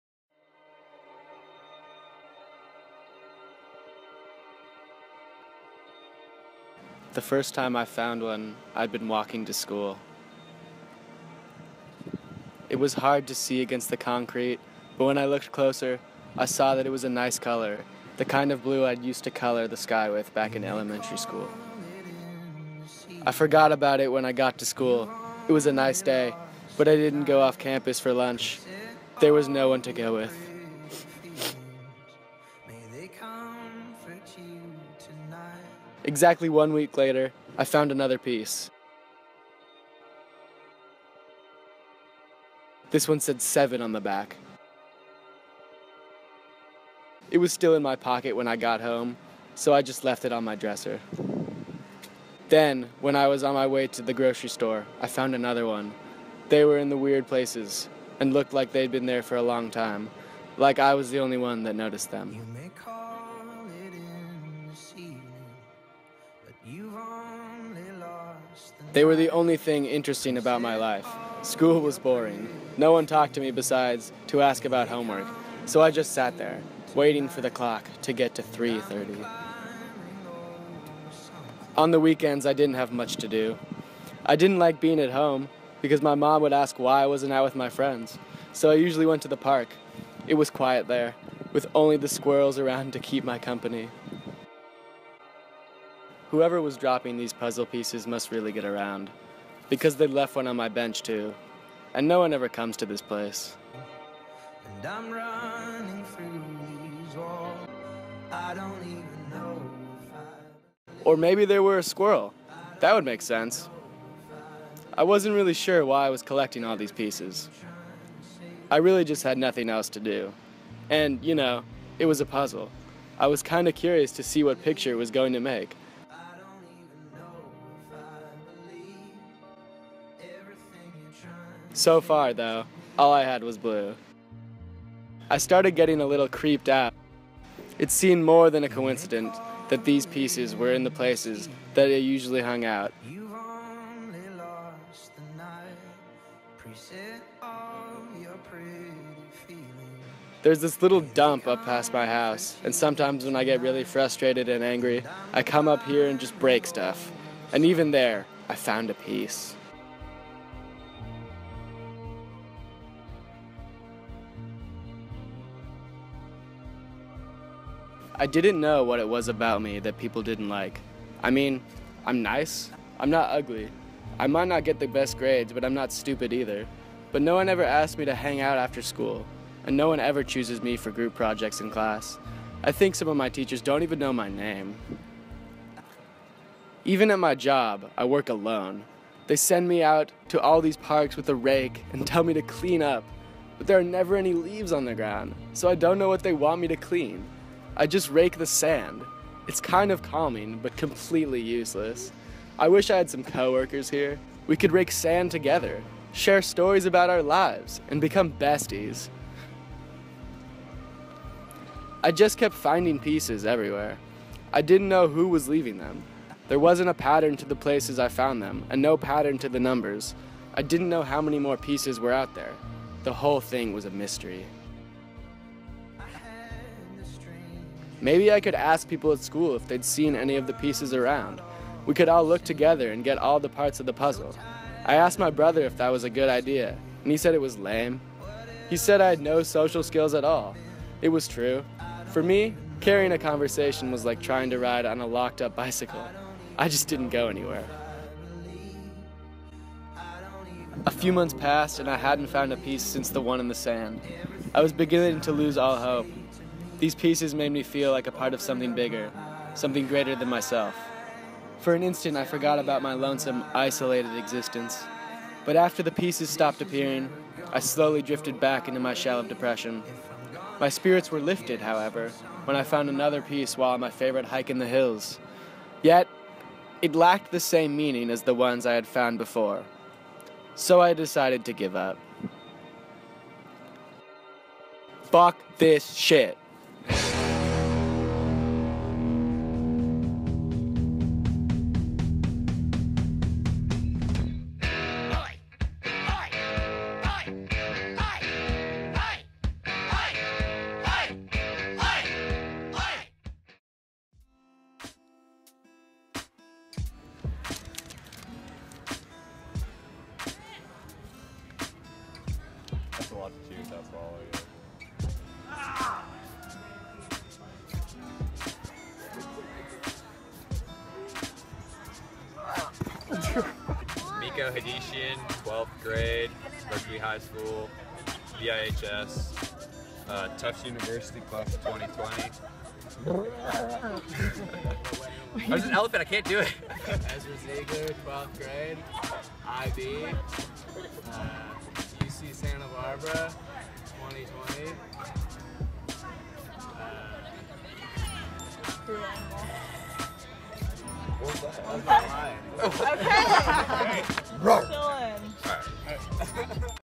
Pieces of Sky - Short Film
A student finds some pieces of sky. For IB English, senior year.